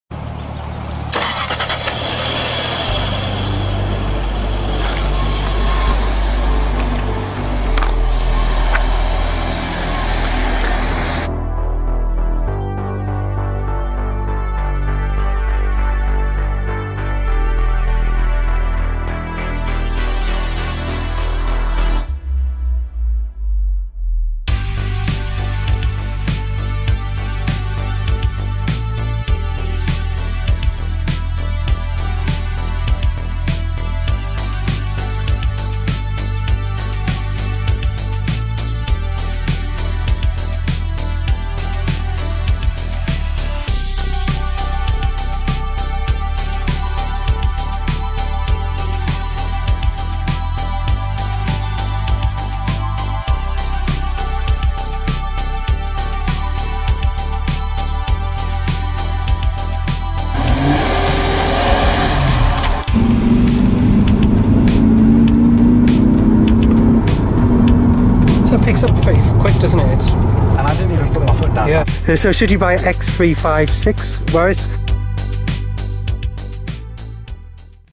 Should You Buy A Jaguar X356? (2006) | 4.2 V8 | Teaser | Full Review Coming Soon!
Should you buy a Jaguar X356?

Quick preview and walkaround to wet your appetite!

Need help with your revision (GCSE to degree level) check out my revision guide.